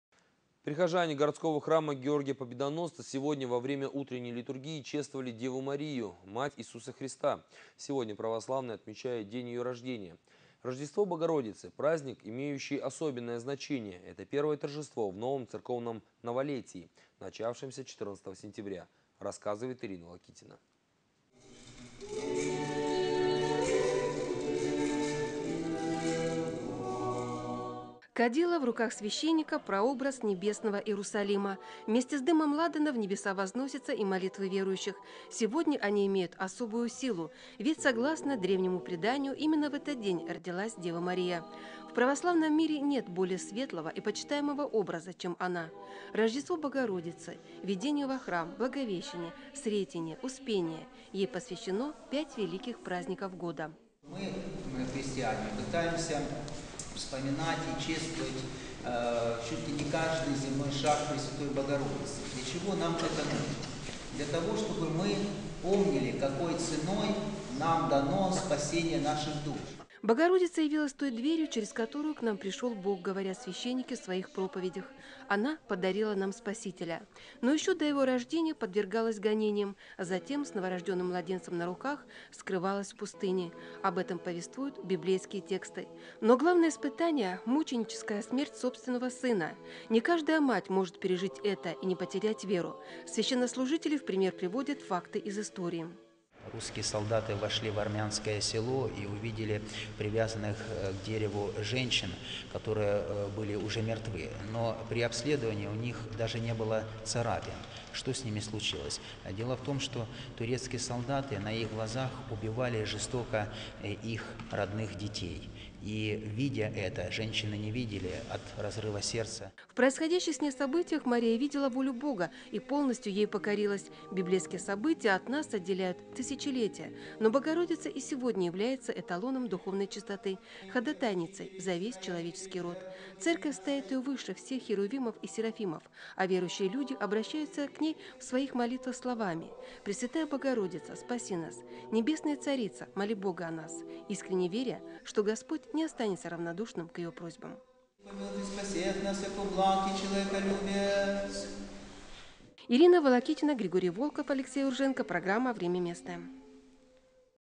Величайшая женщина всех времен
Сегодня православные христиане отмечают Рождество Богородицы.